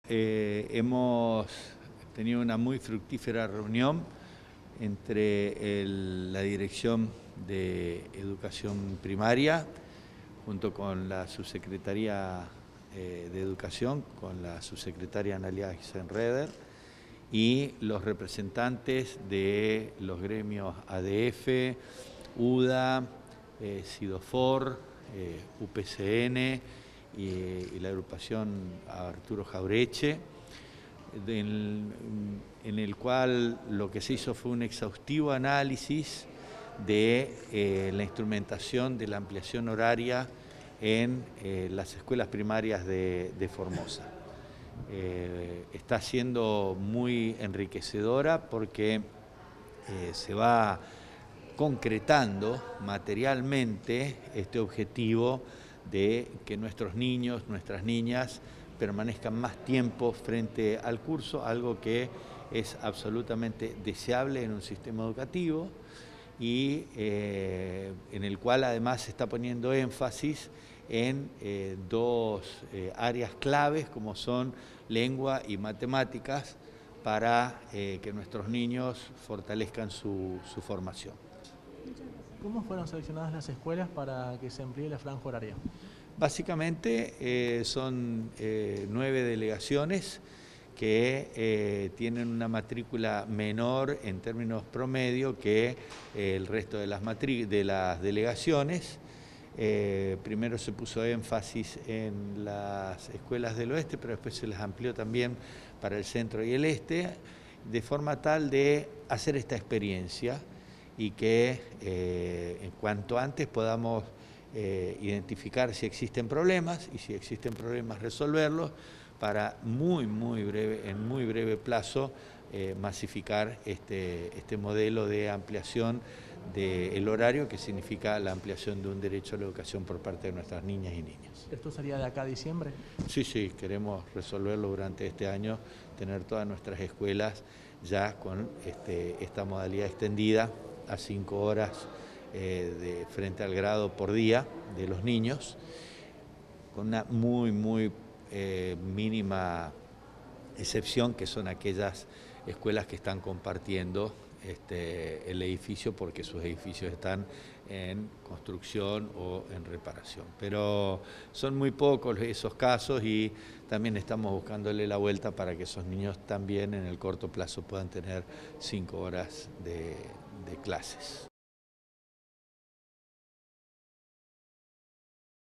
SE ANALIZÓ, CON LOS GREMIOS DOCENTES, EL HORARIO EXTENDIDO EN LAS ESCUELAS
EL MINISTRO DE CULTURA Y EDUCACIÓN, LUIS BASTERRA, SE REUNIÓ CON FUNCIONARIOS DEL ÁREA Y REPRESENTANTES DE GREMIOS DOCENTES, A FIN DE ANALIZAR LA IMPLEMENTACIÓN DEL HORARIO EXTENDIDO EN ESCUELAS PRIMARIAS DE NUEVE DELEGACIONES ZONALES DE LA PROVINCIA.